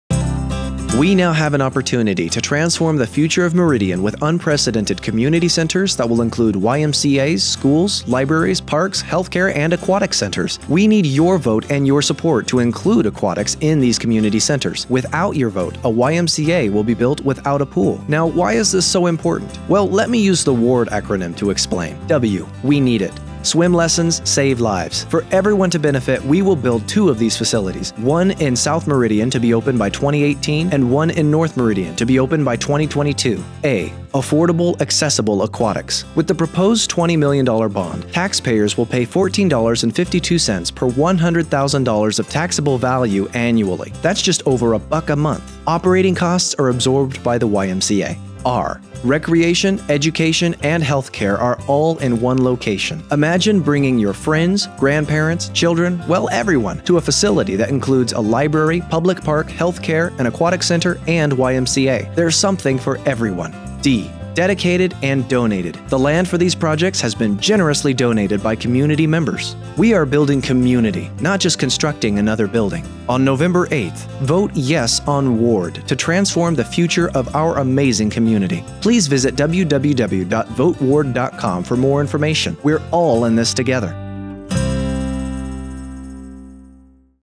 WARD 720p YouTube
Quick summary of the WARD bond election (Meridian, Idaho) offering on election day November 8th, 2016.  Overview of the larger vision of the projects and promotion of adding pools and libraries to these amazing centers.